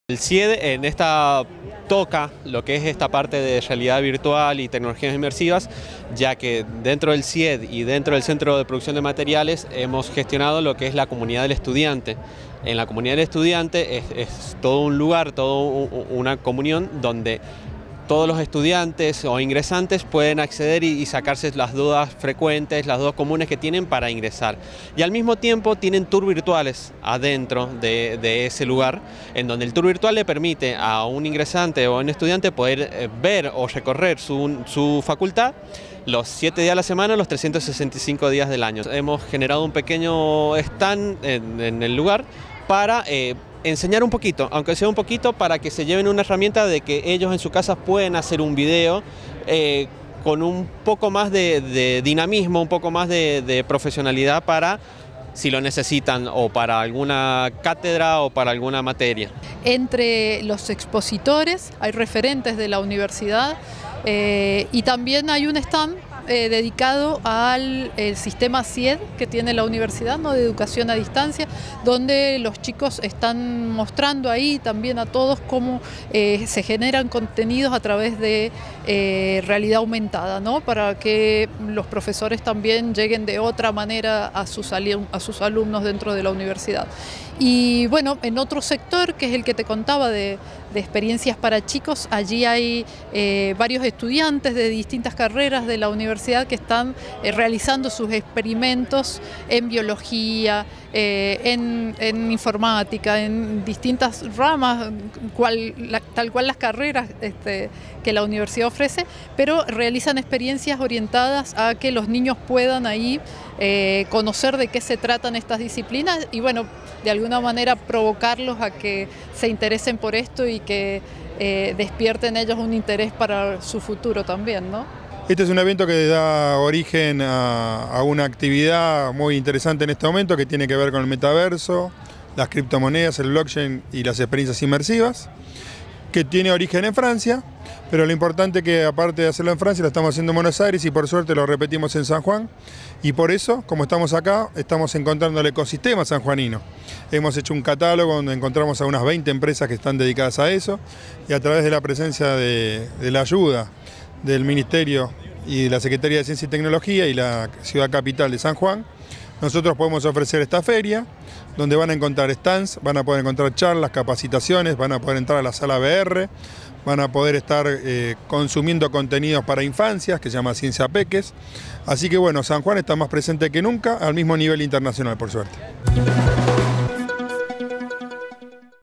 La feria "Virtuality" se desarrolló con éxito 08/08/2022
Entrevistados/as:
- Sebastián Godoy, coordinador del Centro de Producción de Materiales Educativos SIED.
- Mariela Limerutti, coordinadora de Innovación y Creatividad SECITI.
- Gabriel Giandinoto, organizador.